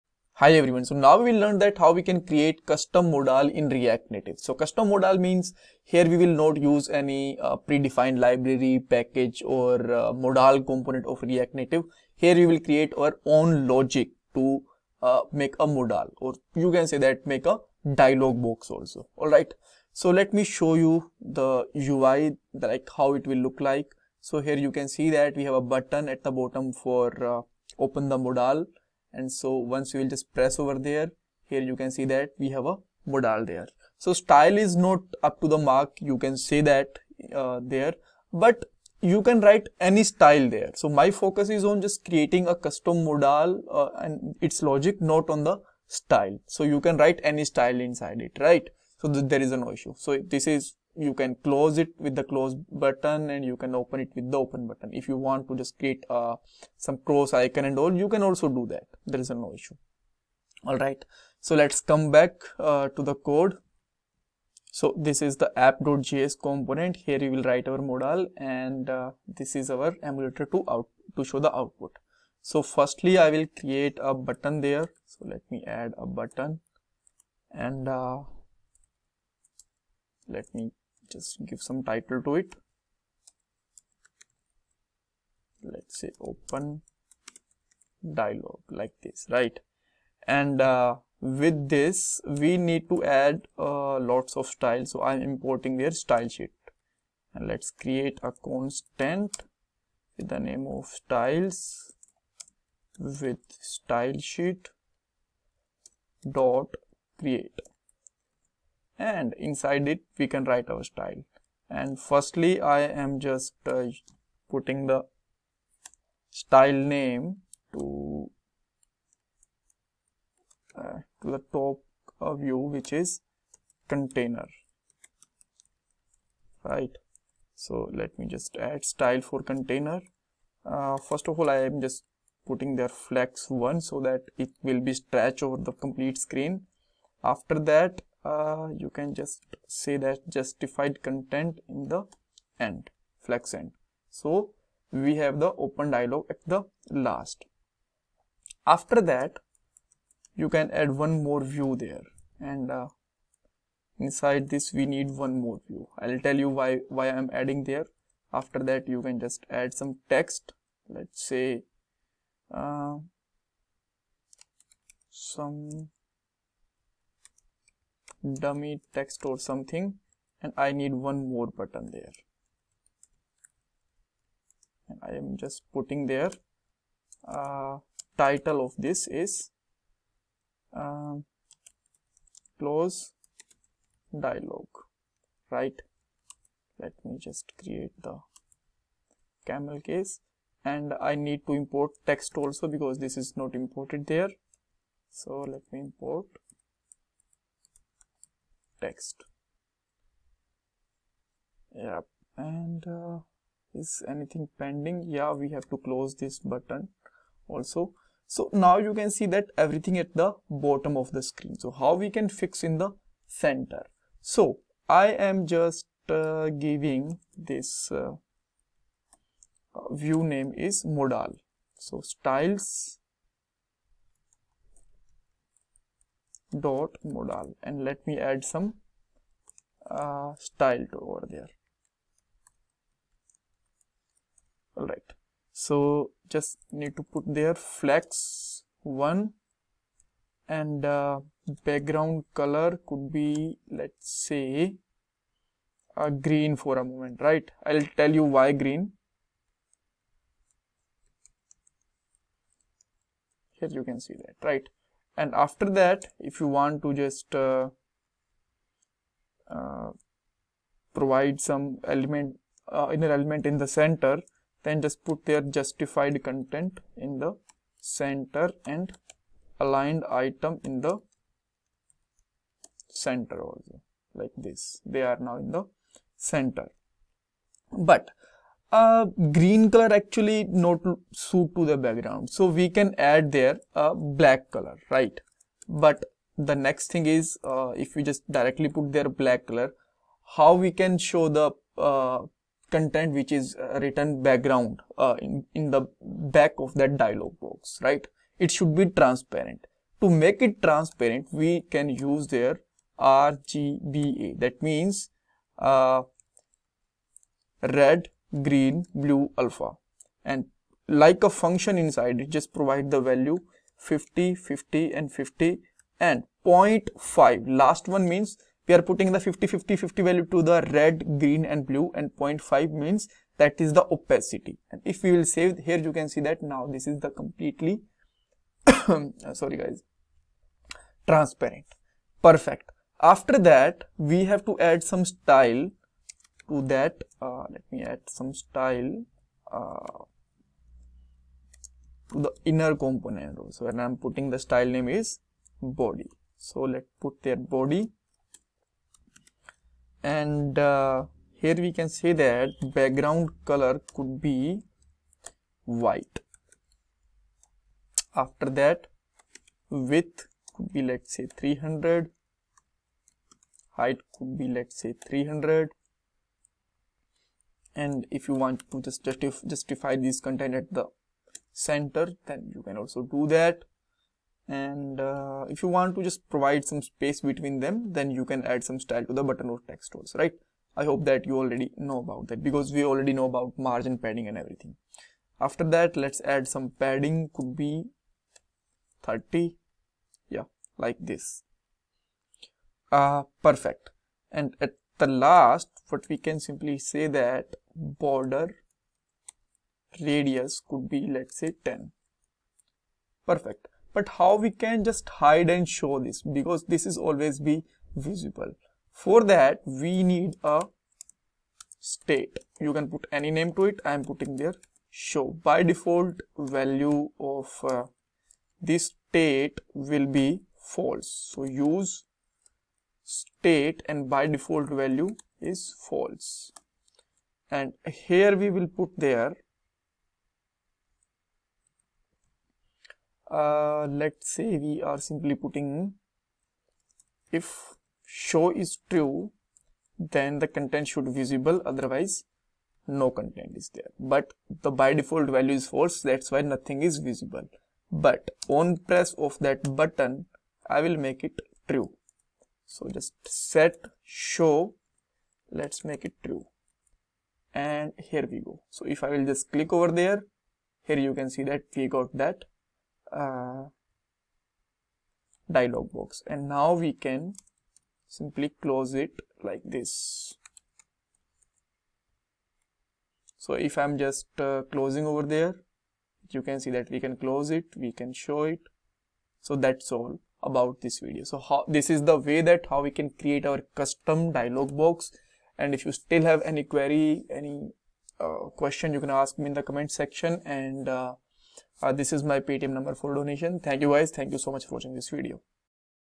React Native tutorial #37 Make Custom Modal Dialog box
Instragram Id : code.steps
Here we will learn about how to make a custom modal in React native and Check OS in react native English beginners series. This React native course is made by anil Sidhu in the English language.

Add Button and UI component
Use Style to Make Modal
Show and Hide Modal with state
Interview Question